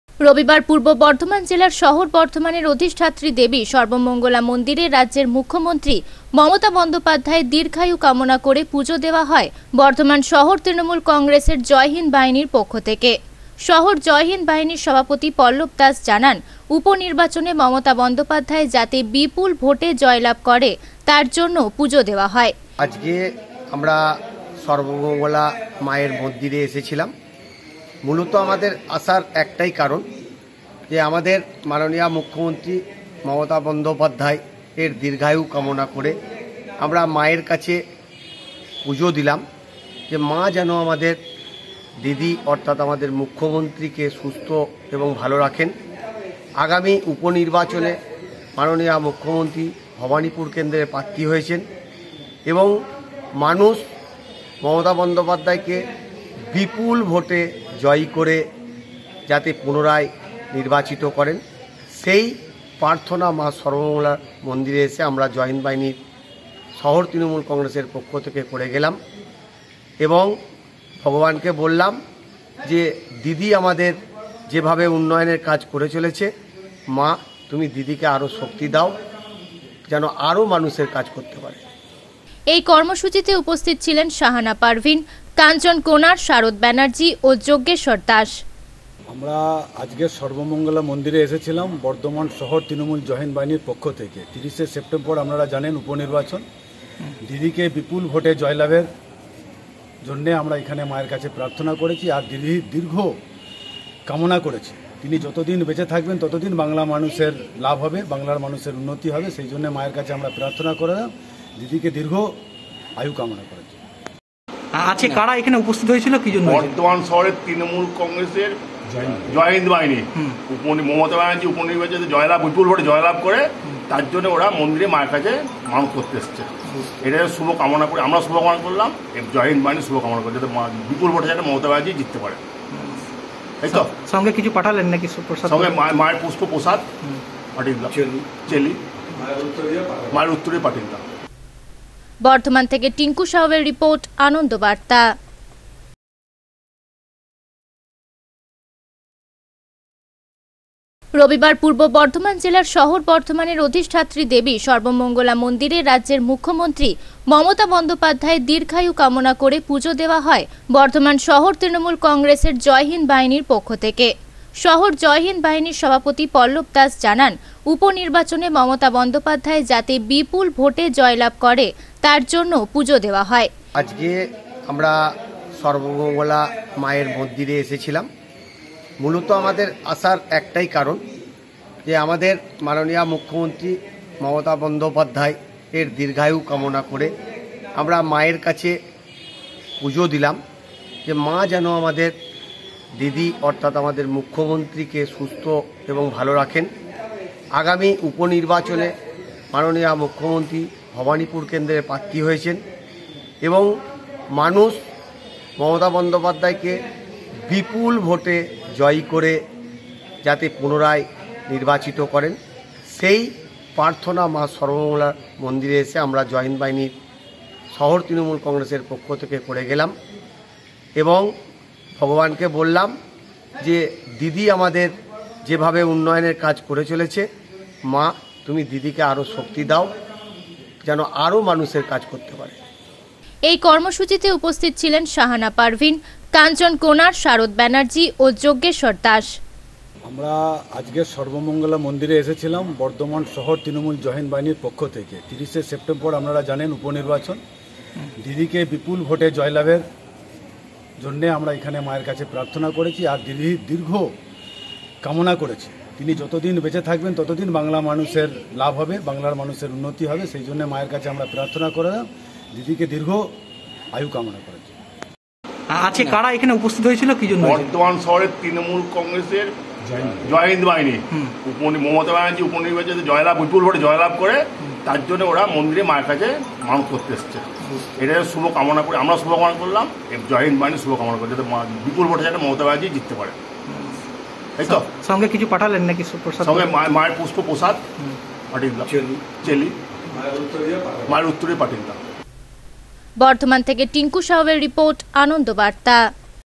মুখ্যমন্ত্রীর দীর্ঘায়ু কামনা করে জয় হিন্দ বাহিনীর পক্ষ থেকে পূজো দেয়া হলো
পূর্ব বর্ধমান জেলার শহর বর্ধমানের অধিষ্ঠাত্রী দেবী মা সর্বমঙ্গলা মন্দিরে জয় হিন্দ বাহিনীর পক্ষ থেকে পূজো দেয়া হলো মুখ্যমন্ত্রীর দীর্ঘায়ু কামনা করে